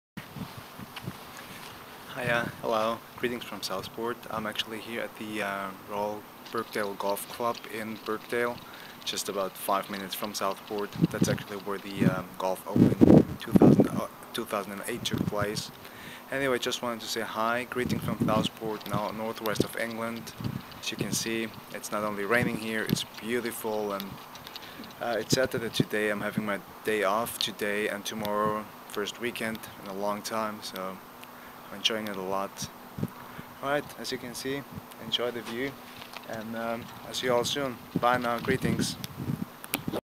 Ray at the Royal Birkdale Golf Club
This is the Royal Birkdale Golf Club. Venue for the Golf Open 2008